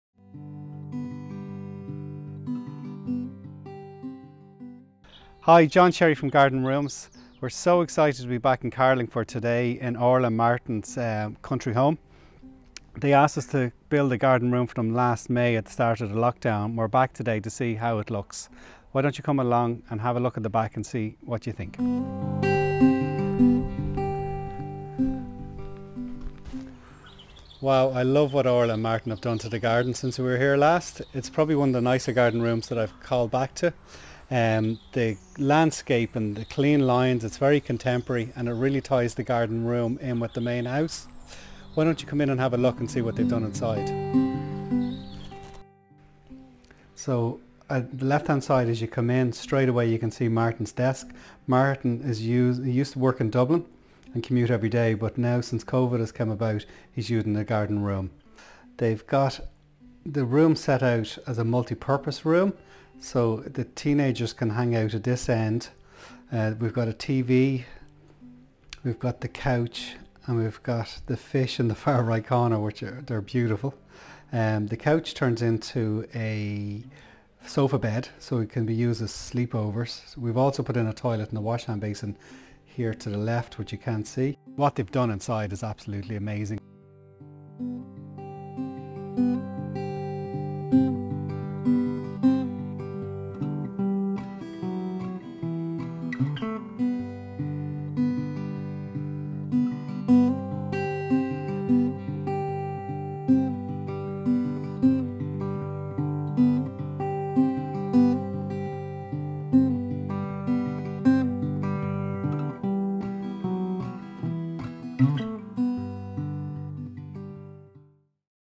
Beautiful Ultimate 23 Garden Room in Carlingford, Co Louth
Watch along as John Sherry revisits this beautiful Ultimate 23 we built for Orla and Martin at their country home in Carlingford, Co Louth just after lockdown began in May 2020.

The landscaping around the Garden Room is absolutely amazing with its clean lines, and just ensures the Garden Room sits perfecting in the Garden.

Once inside you will notice the Home Office section on the left hand side.  This is where Martin works from during the day.  Prior to Covid, Martin used to commute daily to Dublin.

On the right hand side we have a couch and TV for some down time.  The couch also converts to a sofa bed for when the teens wish to have sleepovers.  There is also a toilet and wash hand basin in this Garden Room.

We think that the internal decor in this Garden Room is just beautiful.